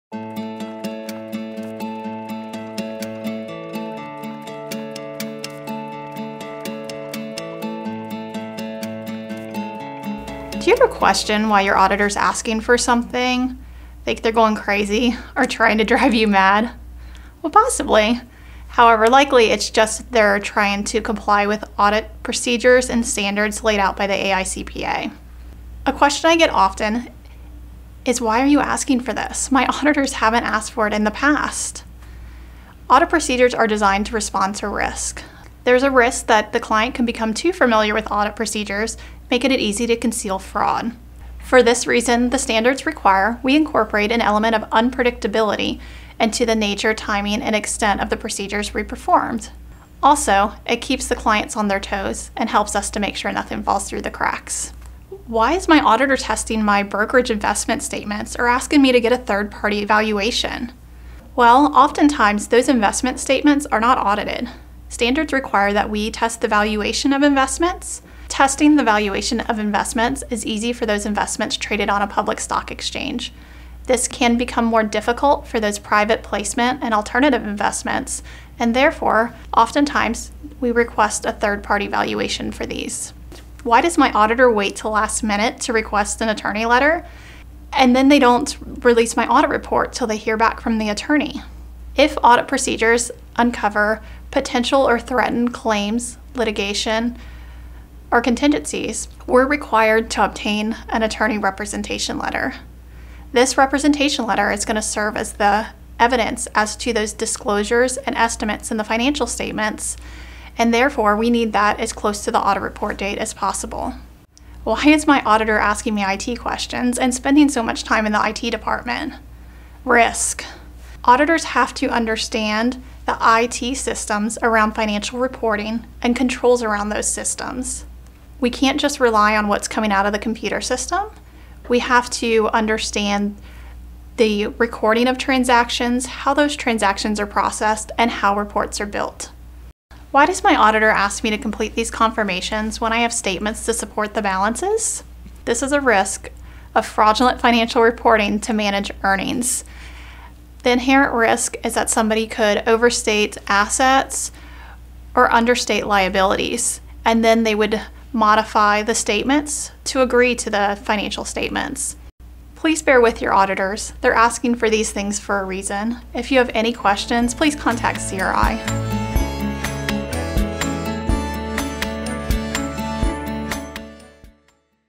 What is My Auditor Doing? 5 Sane Reasons For The Madness
Do you ever wonder why your auditor may be asking you for something you've never had to provide before? Cheri Swain explains how CRI auditors cover their bases to allow you to easily respond to risk and ensure that they are staying compliant with audit procedures laid out by the AICPA.